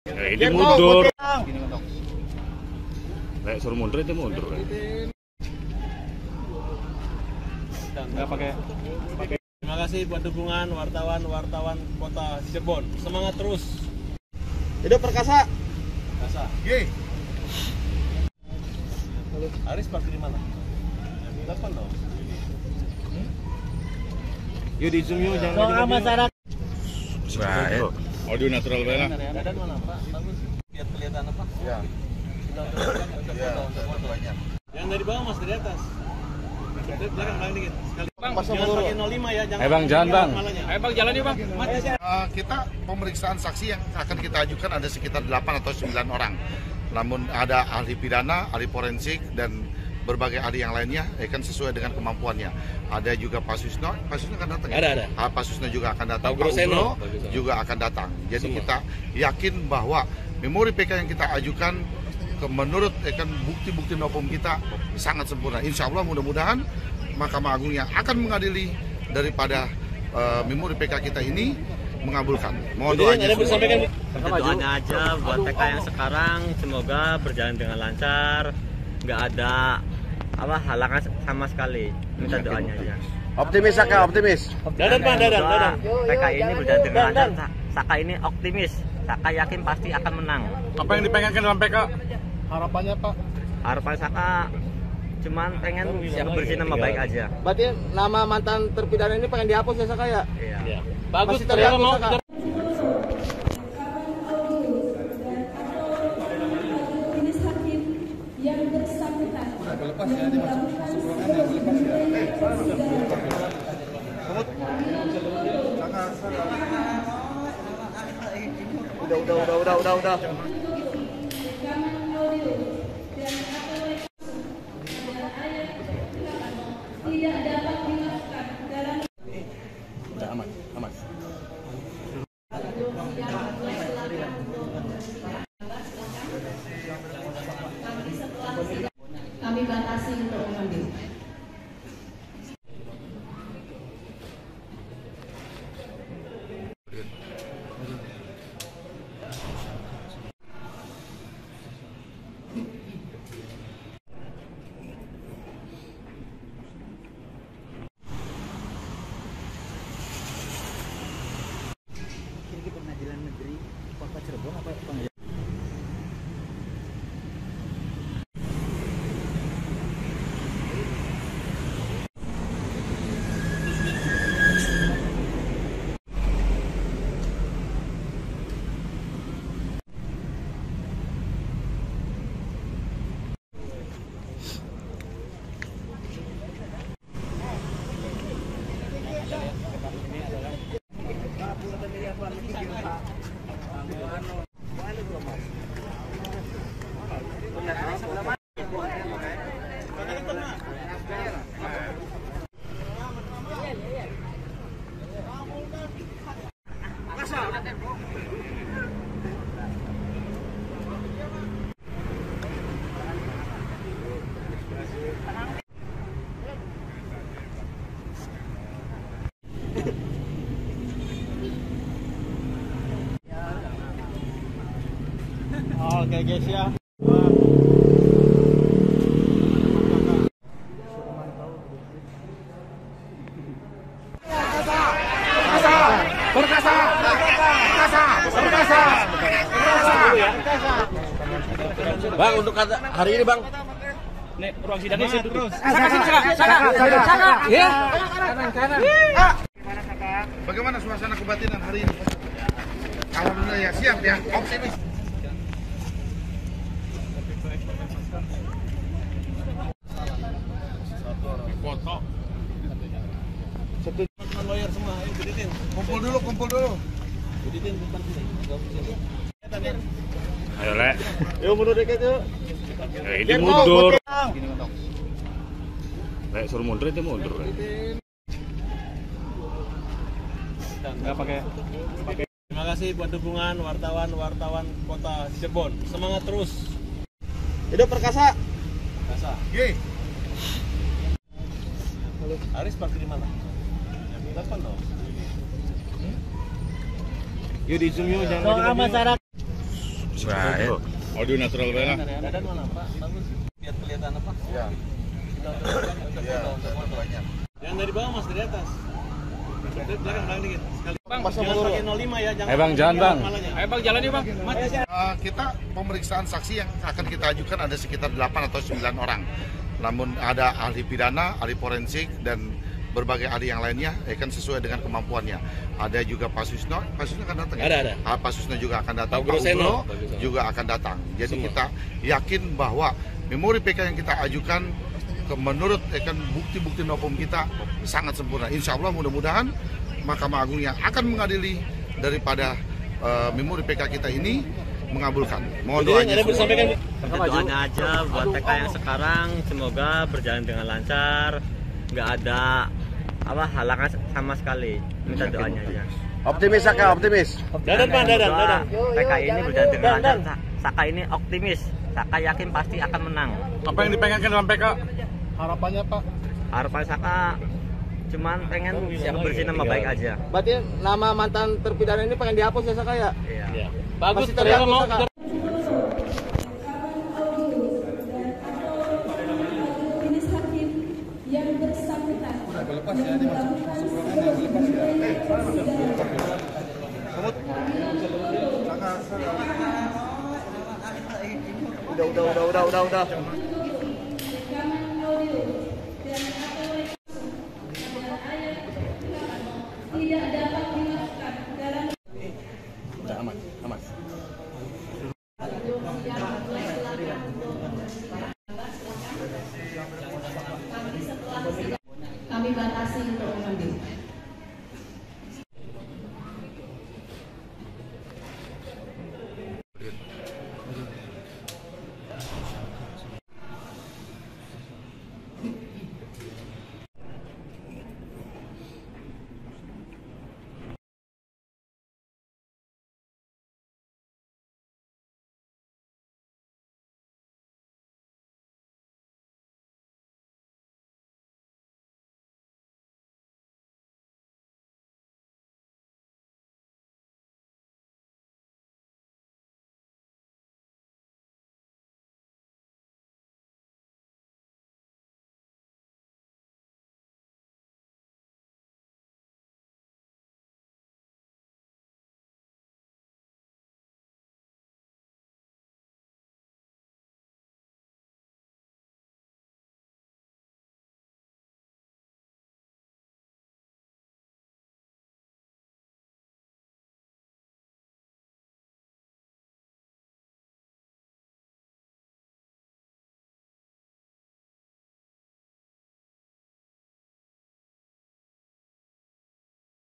Detik-Detik Saka Tatal Beserta Kuasa Hukum Tiba di Pengadilan Negeri Cirebon
Saka Tatal bersama tim kuasa hukumnya tiba di Pengadilan Negeri (PN) Cirebon, Jawa Barat siang tadi, Rabu (24/7/2024) untuk menghadiri sidang peninjauan kembali (PK) perdana.

Saka Tatal optimistis bisa menangkan PK dan membersihkan nama baiknya. Adapun sidang perdana PK Saka dijadwalkan untuk pembacaan memorandum PK serta legal standing para kuasa hukumnya.

Para pengacara juga telah menyiapkan sembilan saksi dan delapan bukti baru untuk memperkuat kasus mereka.

Kedatangan Saka dan tim kuasa hukumnya pun disambut dukungan dari warga Saladara di depan Pengadilan Negeri Cirebon.